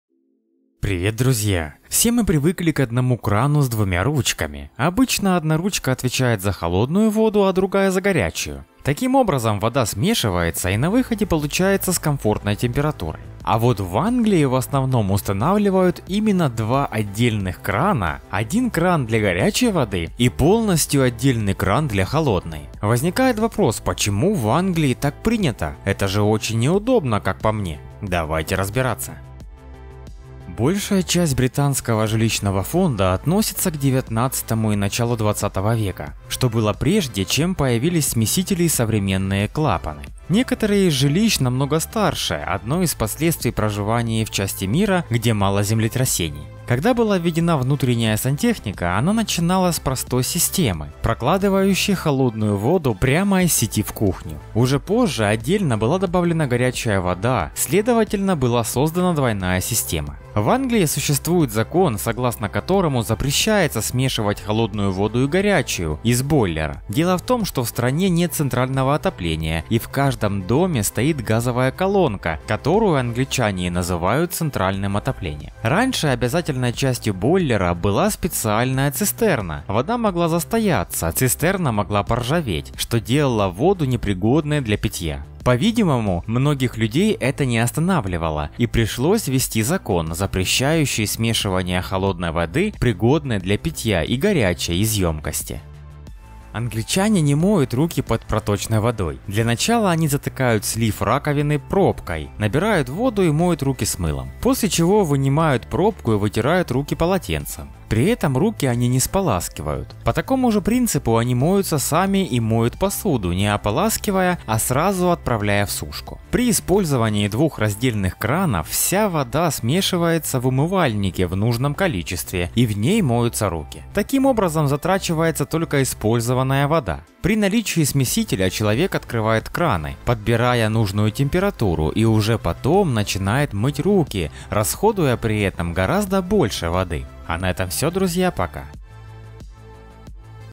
Почему в Англии используют два отдельных крана для горячей и холодной воды?
Привет, друзья. Все мы привыкли к одному крану с двумя ручками. Обычно одна ручка отвечает за холодную воду, а другая за горячую. Таким образом вода смешивается и на выходе получается с комфортной температурой. А вот в Англии в основном устанавливают именно два отдельных крана, один кран для горячей воды, и полностью отдельный кран для холодной. Возникает вопрос почему в Англии так принято? Это же очень не удобно. Давайте разбираться.